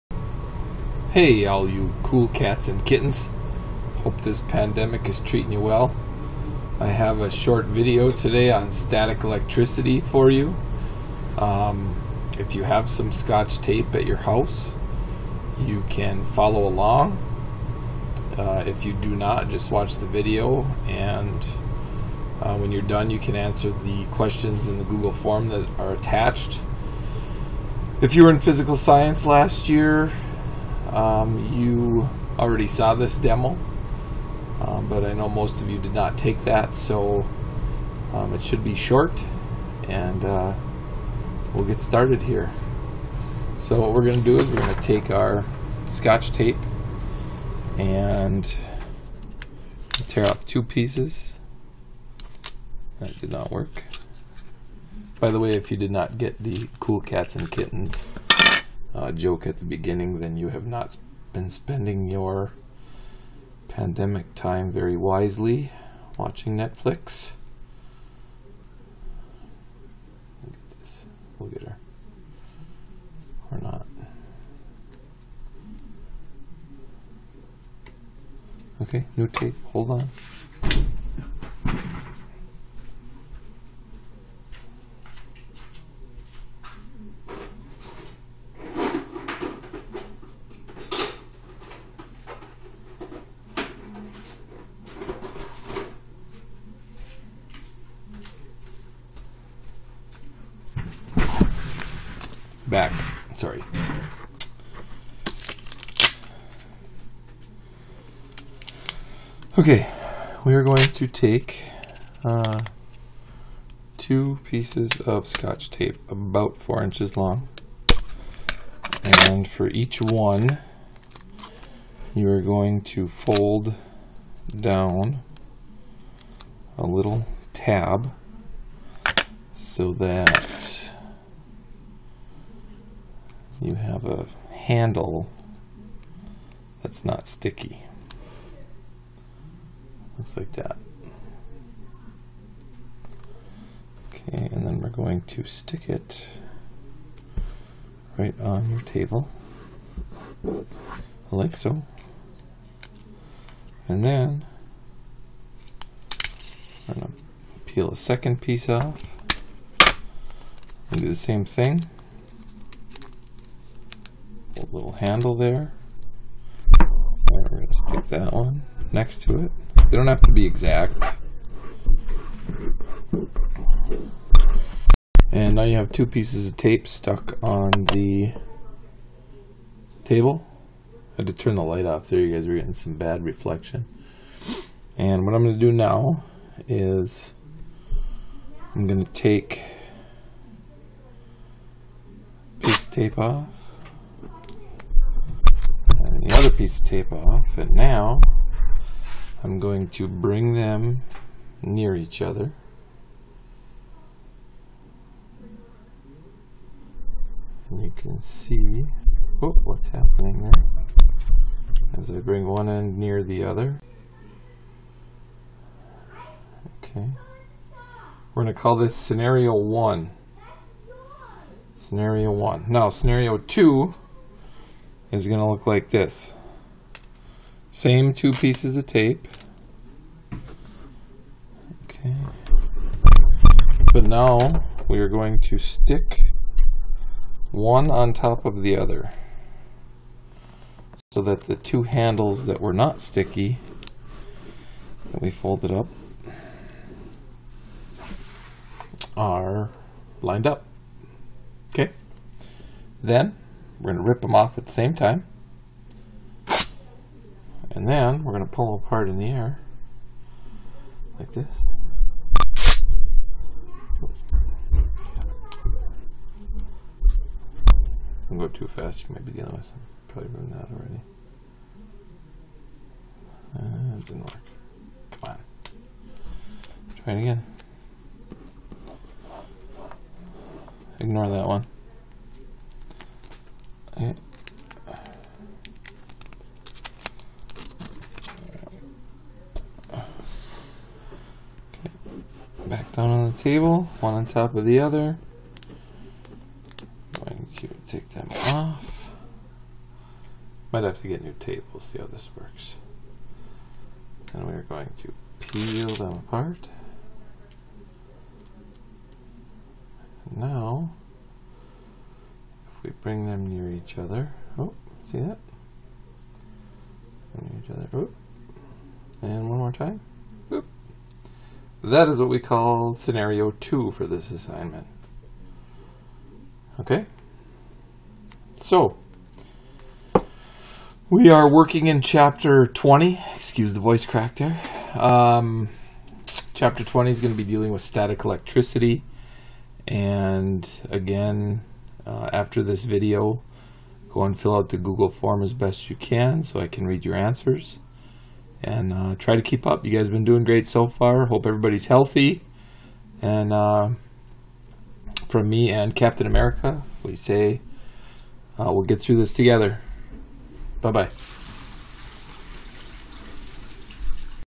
Scotch tape and Static electricity
My first video of myself attempting to do a demonstration with scotch tape and static electricity.  The lighting is terrible!